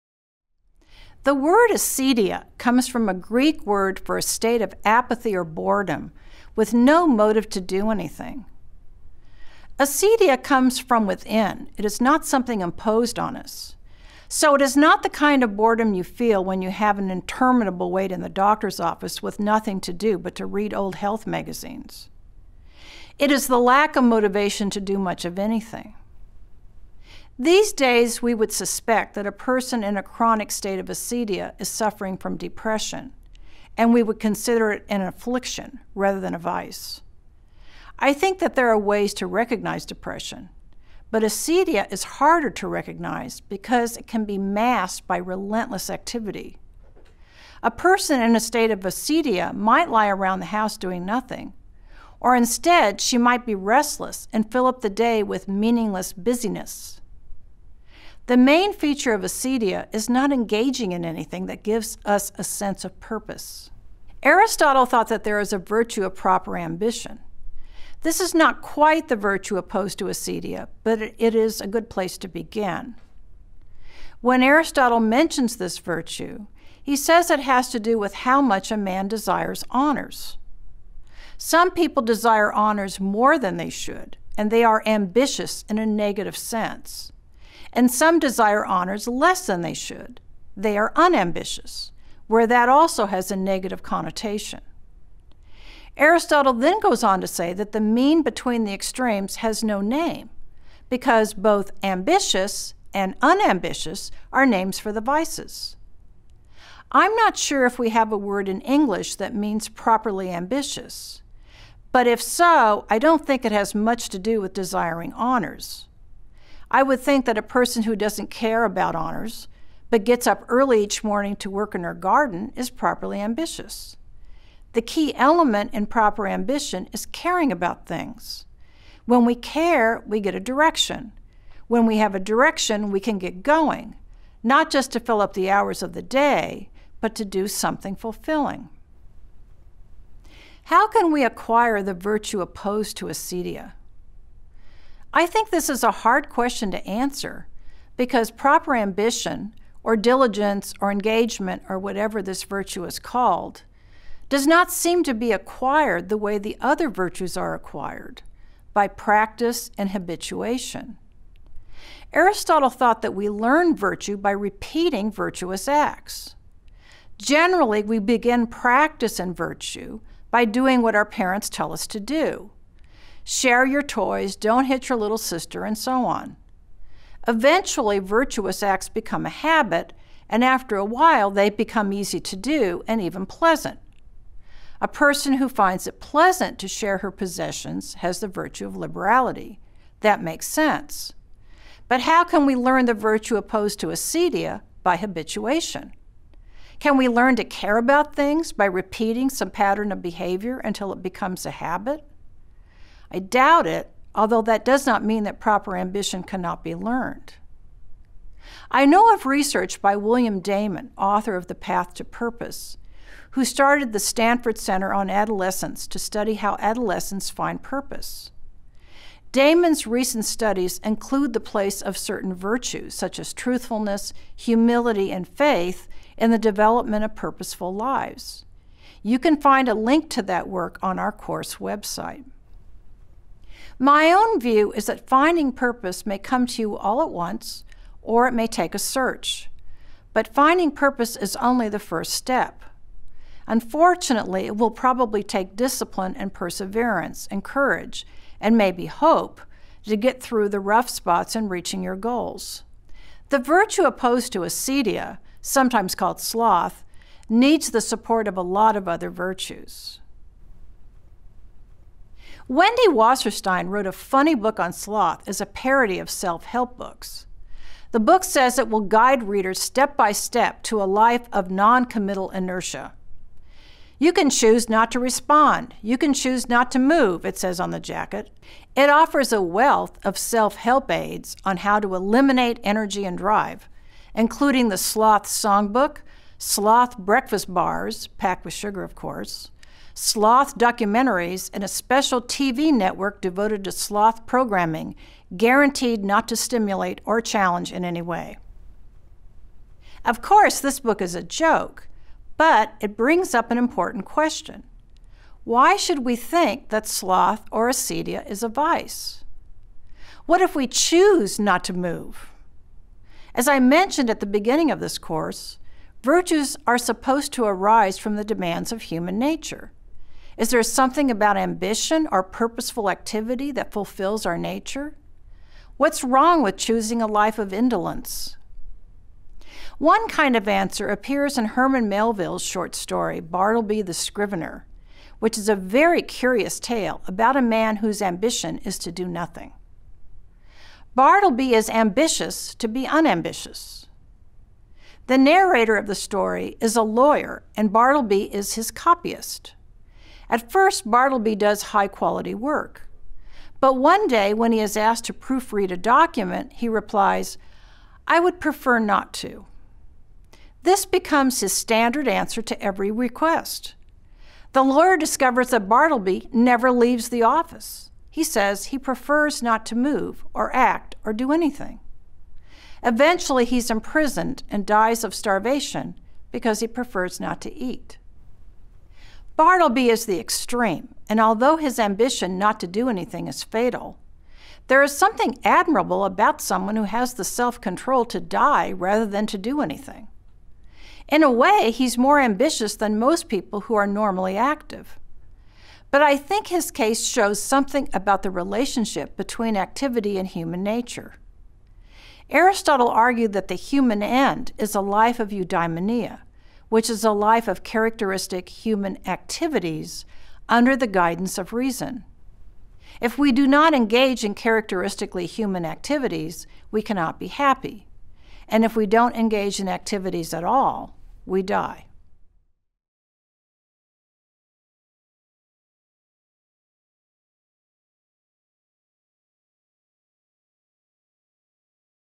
Virtue Ethics | Moral Virtues | Acedia
“Virtue Ethics“ is a free online course on Janux that is open to anyone.

Dr. Linda Zagzebski is a George Lynn Cross Research Professor and the Kingfisher College Chair of the Philosophy of Religion and Ethics at the University of Oklahoma